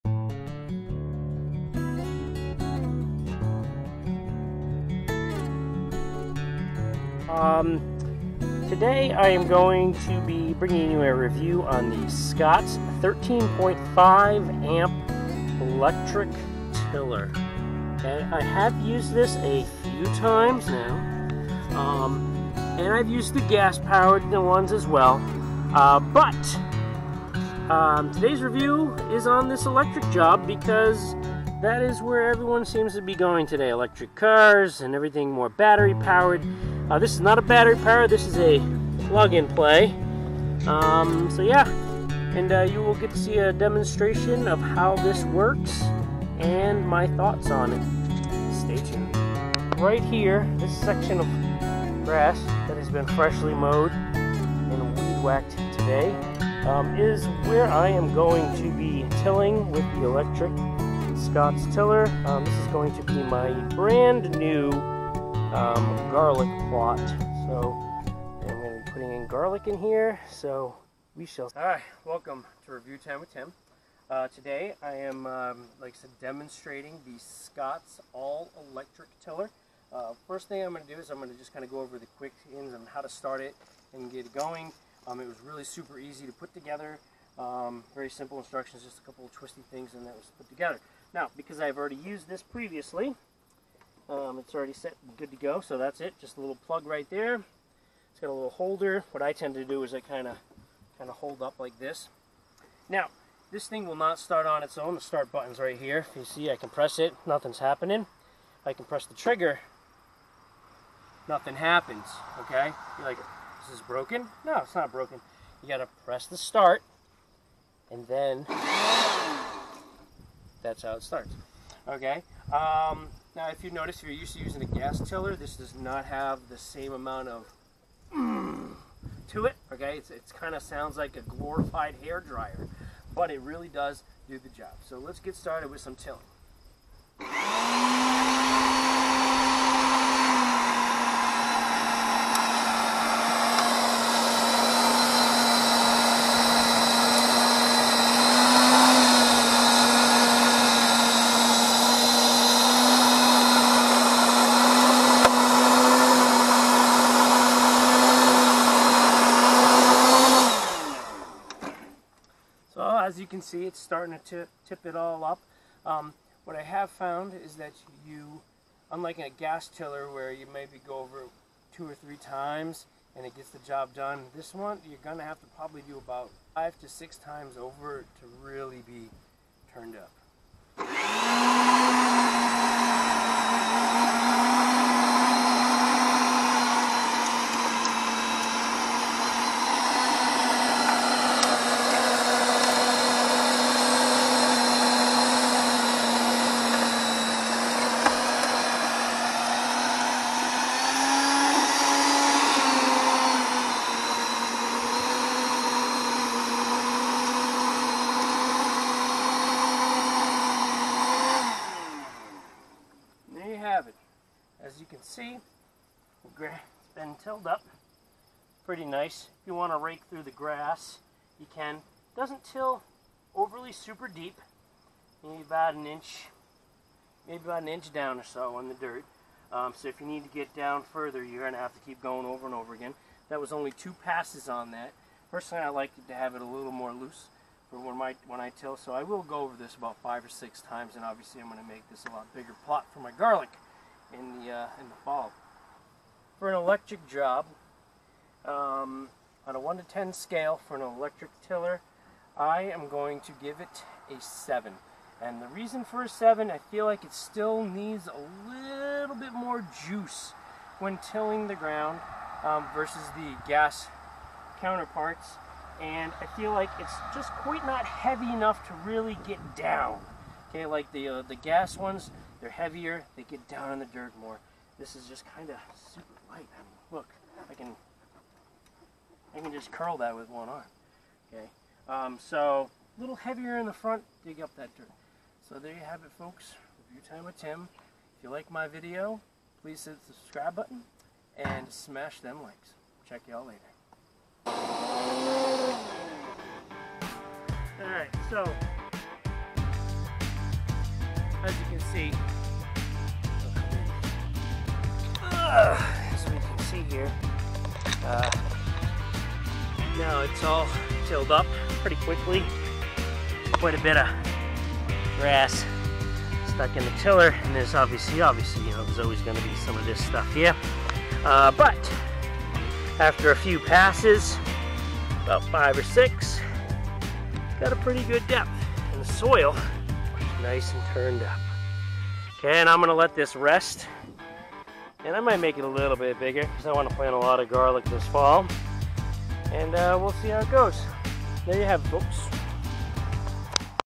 SCOTTS 13.5 Amp TILLER Review #37
Review time with Tim brings you a review on an Electric Scotts Tiller